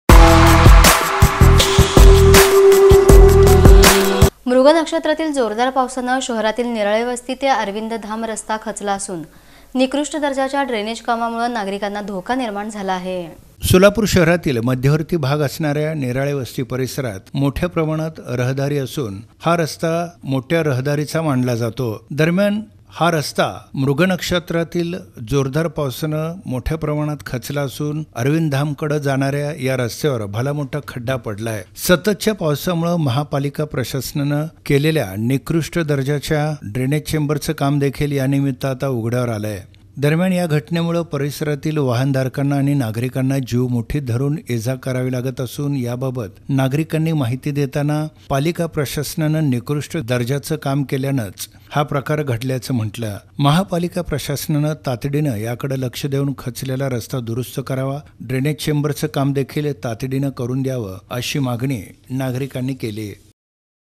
निराळे वस्ती ते अरविंद धाम रस्ता, मृग नक्षत्राच्या जोरदार पावसानं खचला | NIRALE VASTI RASTA KACHALA
निराळे वस्ती ते अरविंद धाम रस्ता... मृग नक्षत्राच्या जोरदार पावसानं खचला